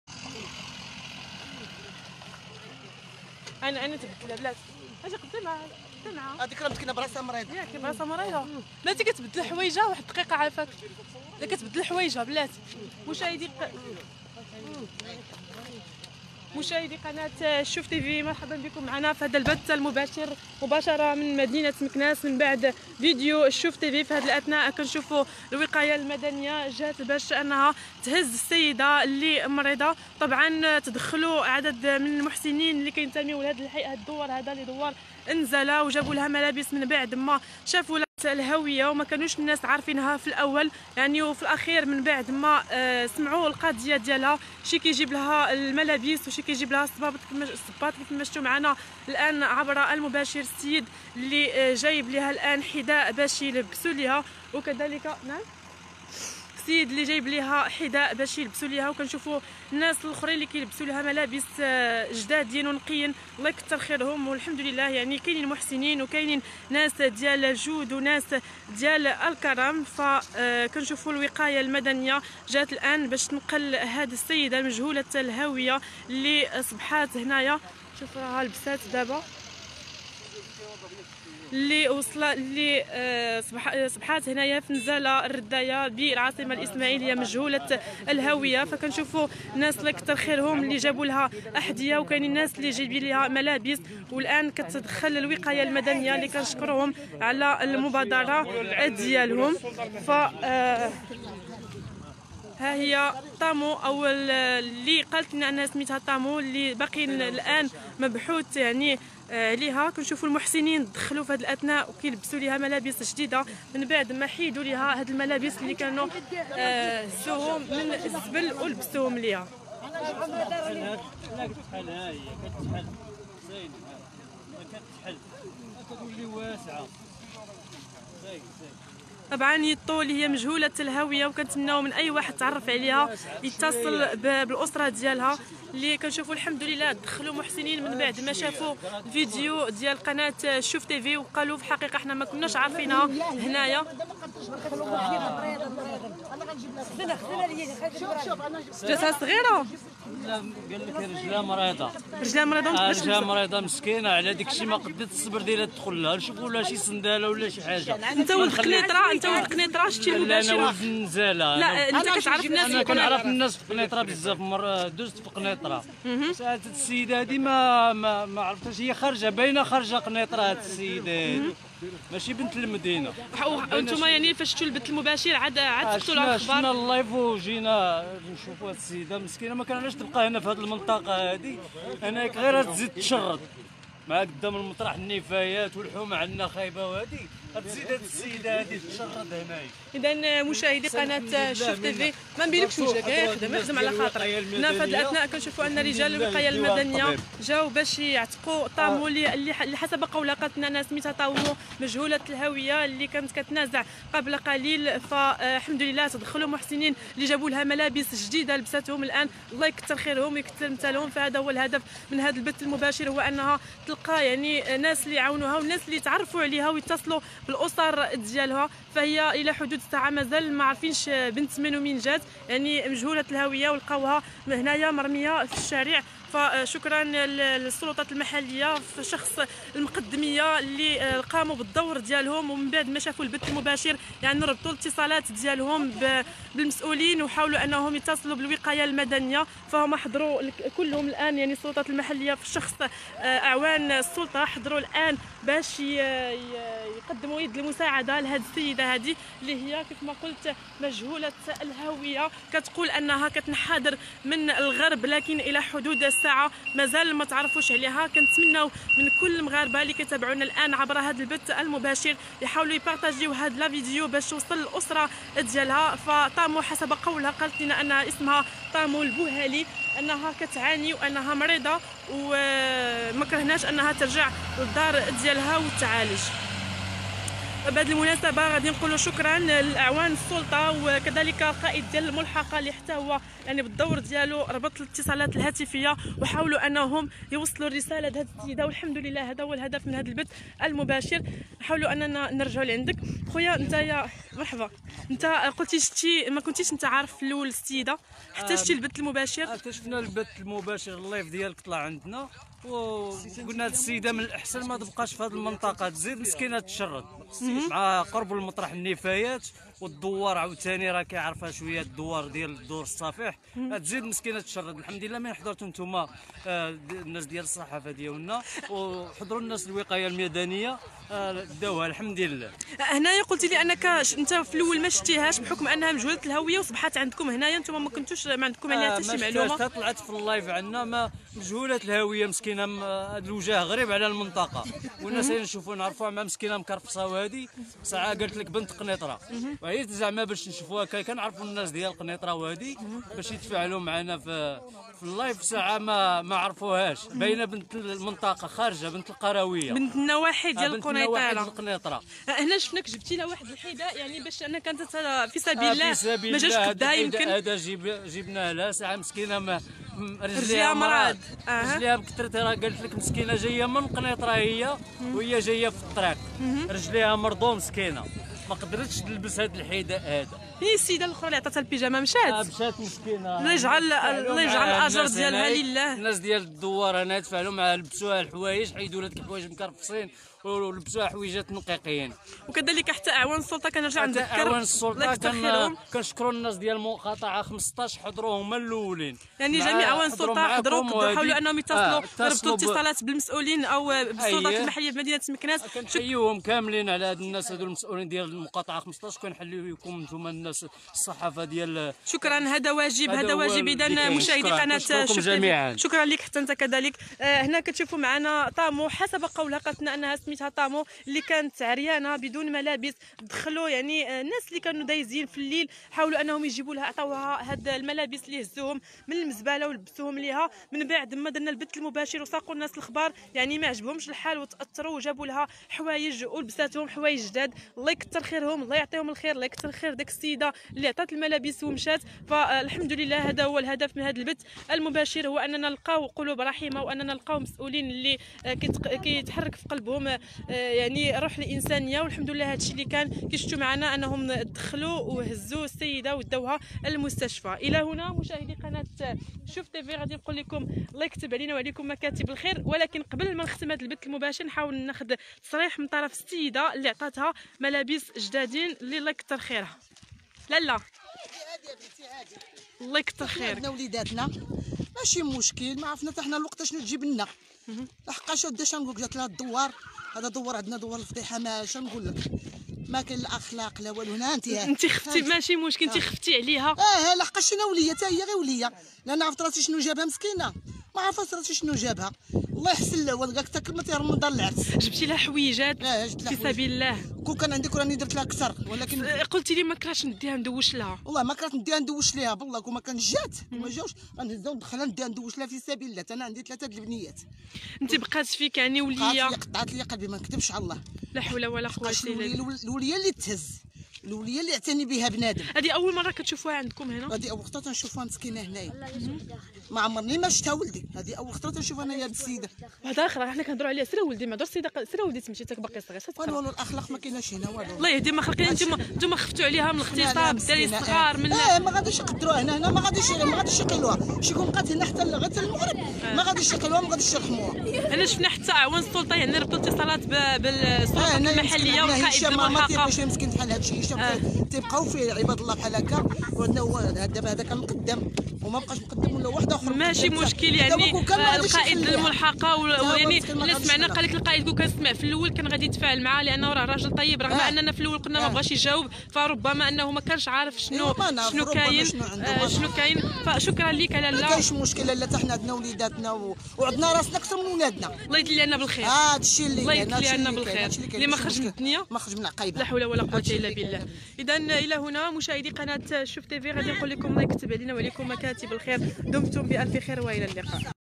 بعد فيديو شوف تيفي البوليس والإسعاف دخلو على خط السيدة لي مجهولة الهوية ولي لقاوها الناس عريانة
عاجل ومباشرة من مكناس:بعد فيديو شوف تيفي البوليس والإسعاف دخلو على خط السيدة لي مجهولة الهوية ولي لقاوها الناس عريانة..شوفو شنو واقع دابا